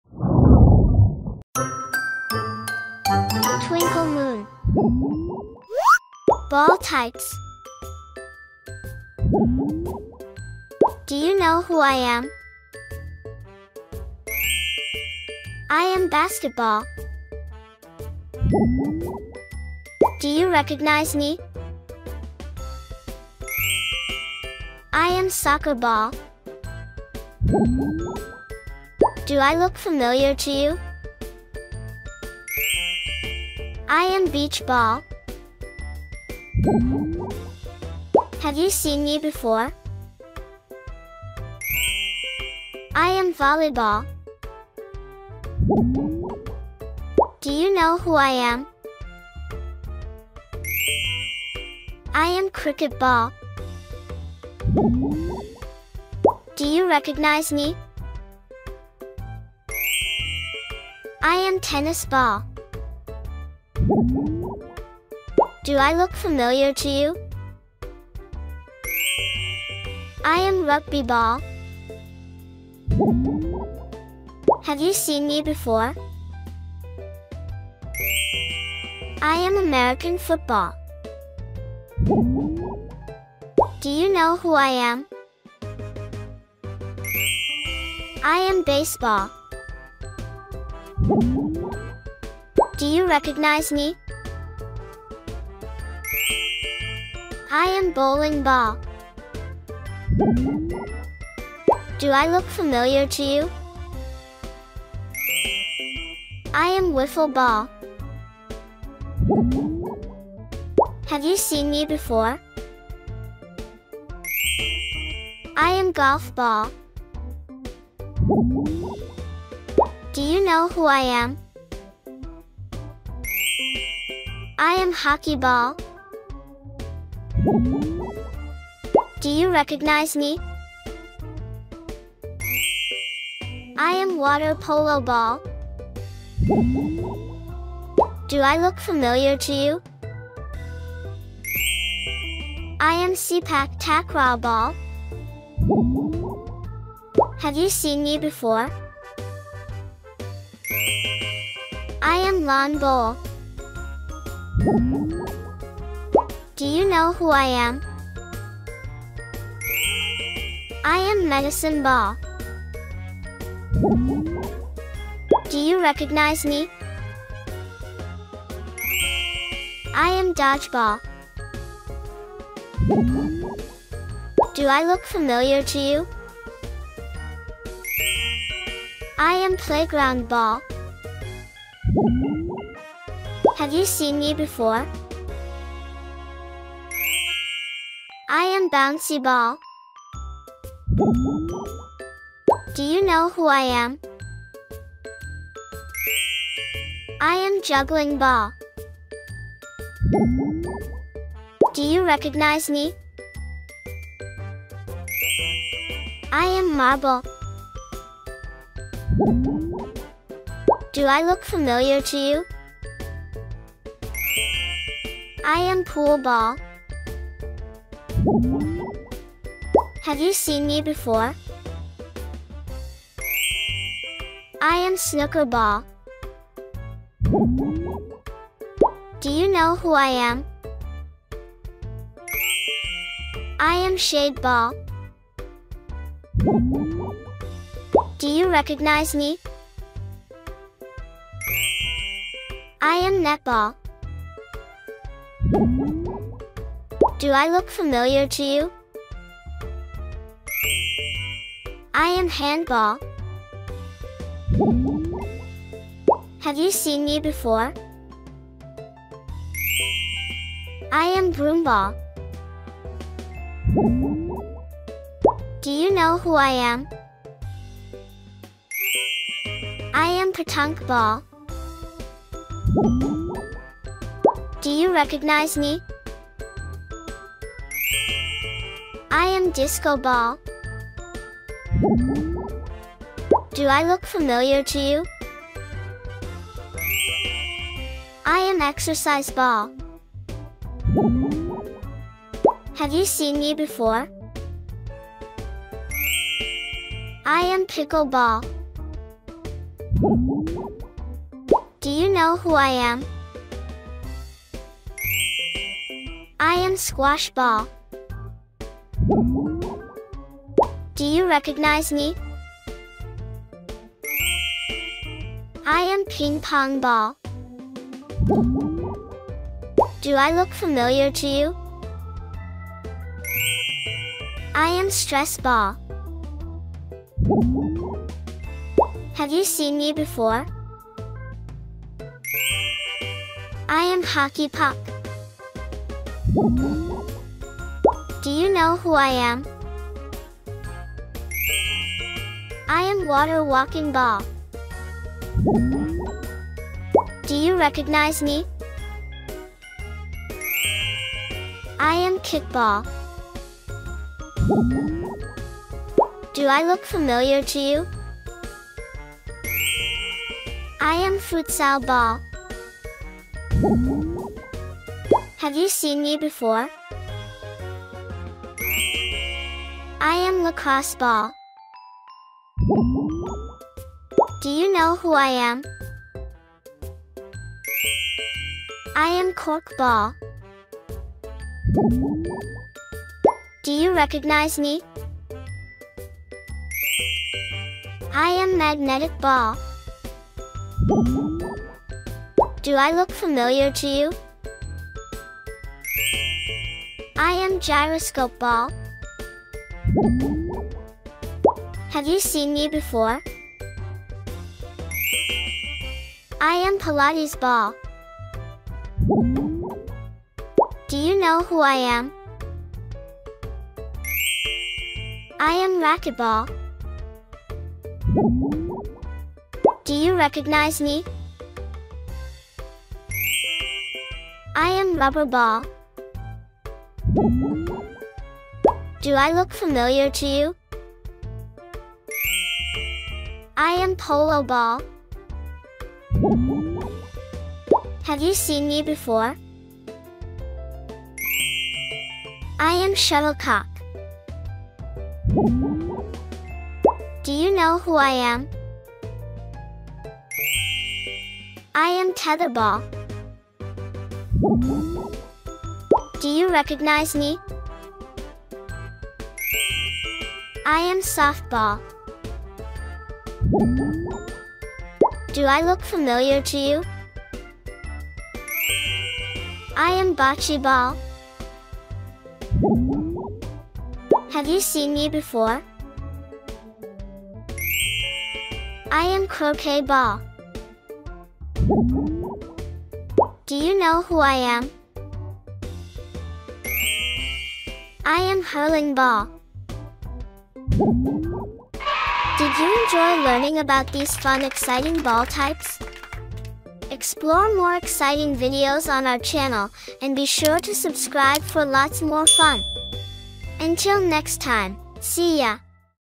50 Ball Types Names for Kids - Learn Ball Types for Children
Are you looking for a fun and educational way to teach kids about different types of balls? Look no further! In this video, we have compiled a list of 50 unique ball types names that are sure to capture the attention of children.

From soccer balls to basketballs, tennis balls to beach balls, we cover it all in this comprehensive guide. Your little ones will have a blast learning about the various shapes, sizes, and materials of these popular playthings.

Not only will this video provide entertainment for the whole family, but it will also help children expand their vocabulary and knowledge of the world around them. So grab a seat, gather the kids, and let's dive into the exciting world of ball types names!